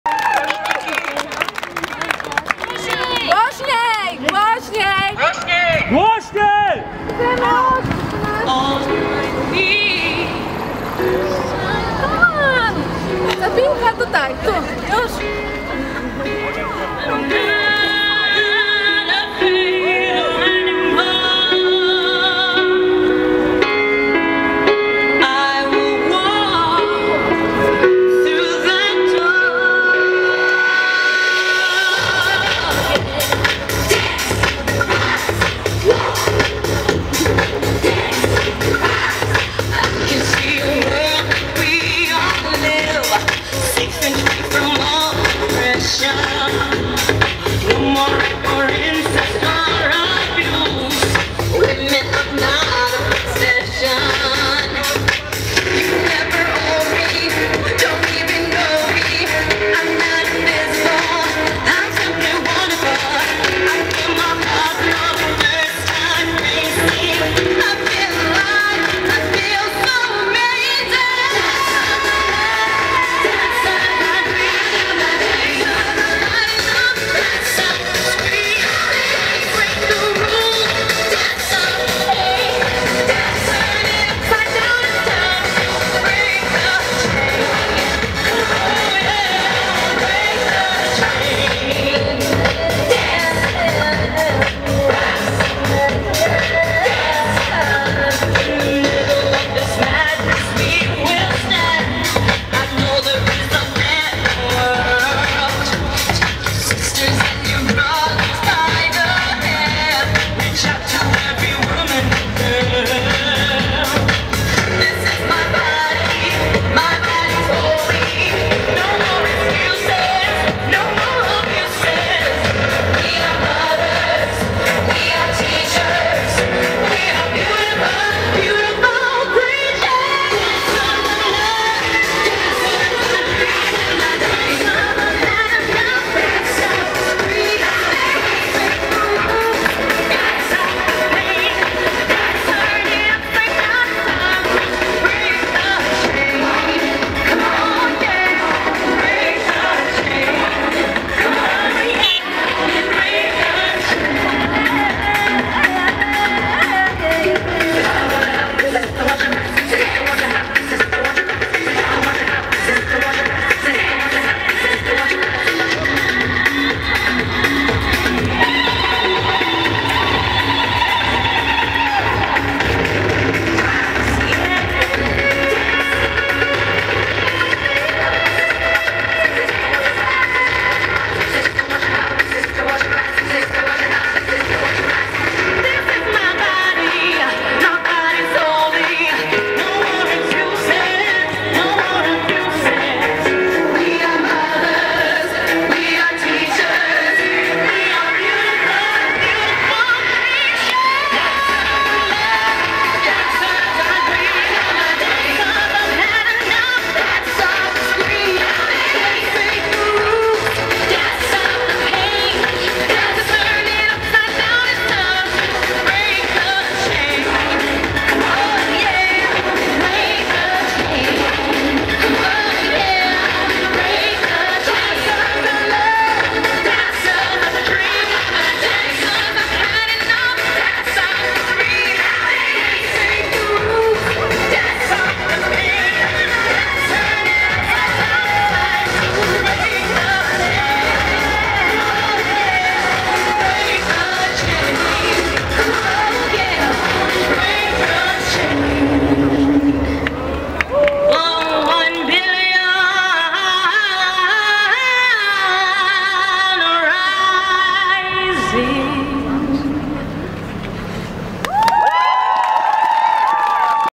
One Billion Rising - Kraków 14.02.2014
Kraków tańczy przeciwko przemocy - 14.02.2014 plac Szczepański godzina 19.00
  One Billion Rising